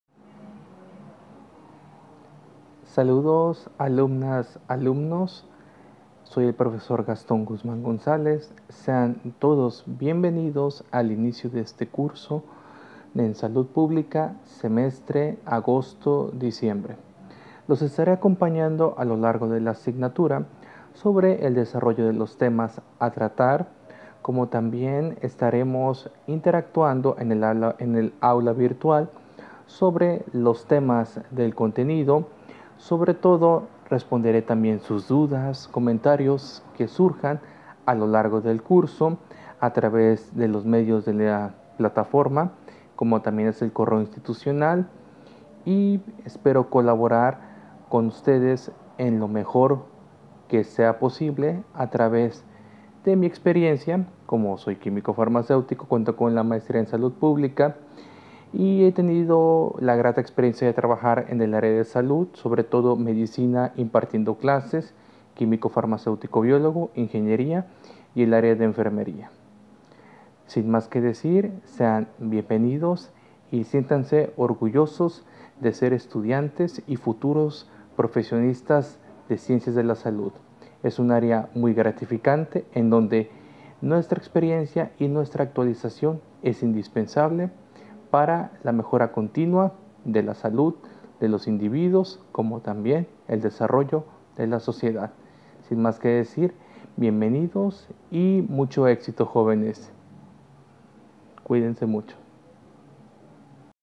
MSP Gastón Guzmán González Mensaje de bienvenida Curso agosto-diciembre Salud Pública UVM Virtual.
Saludos alumnas y alumnos del curso en Salud Pública UVM Virtual semestre agosto - septiembre 2022.